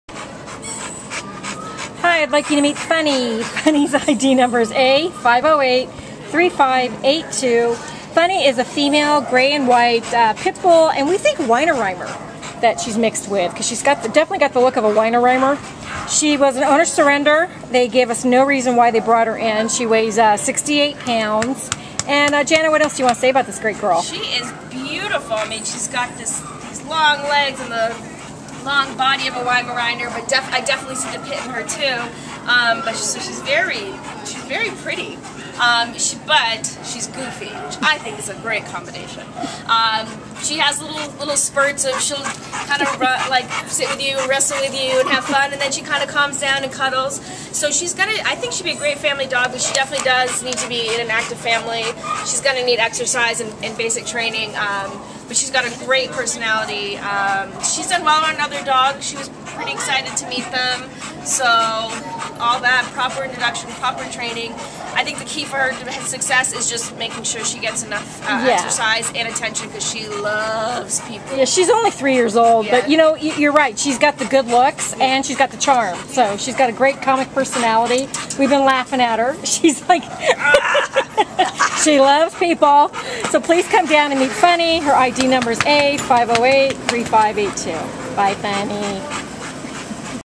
RESCUED! A5083582 Funny | Pit Bull/Weimaraner
A5083582 Funny is an adventurous and charming 3-year-old gray-with-white female Pit Bull and Weimaraner mix whose owner surrendered to the Baldwin Park Animal Care Center on June 27th (we don’t know why). Weighing 68 lbs, Funny is enthusiastic and curious about everything and everyone around her. Funny has adorable gray freckles, different-length white socks, a lovely white chest, and a long white blaze up the center of her face. She’s a strong girl who aced her shelter-administered temperament test, listens well, and is easy to control—even though she pulls a bit on leash, which with a bit of training will likely disappear, turning her into a perfect walking companion. Funny will be happiest as an indoor pet and companion for someone who understands the needs of big dogs and is able to spend the time and effort needed for her to become a perfect canine citizen.